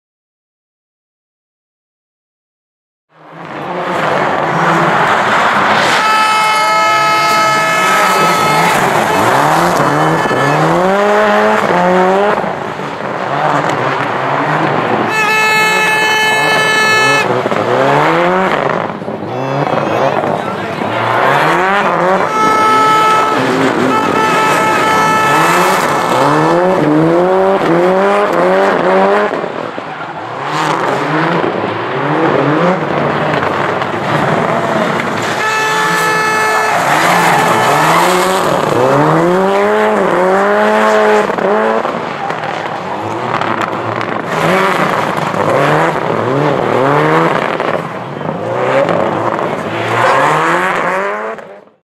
Tomáš Fusko - Subaru Impreza - Dobšinská zima 2016 - Rally
Pilot Tomáš Fusko a navigátor Jakub Slovák si společně dojeli pro 4.místo na amatérské rally Dobšinská zima 2016